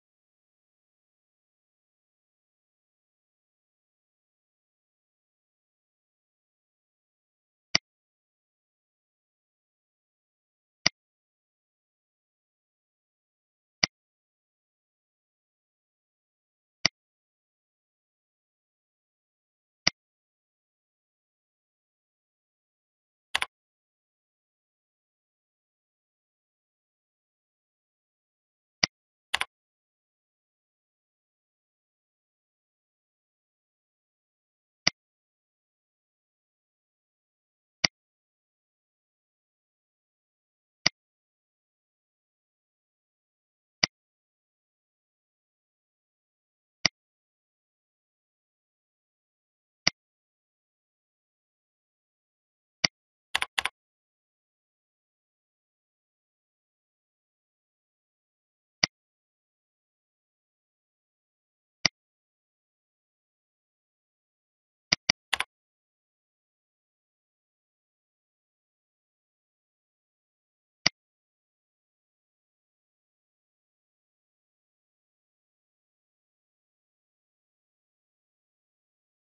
Apply Field Validation in Table
In this demo, we will learn how to apply field validation in table in MS Access 2007.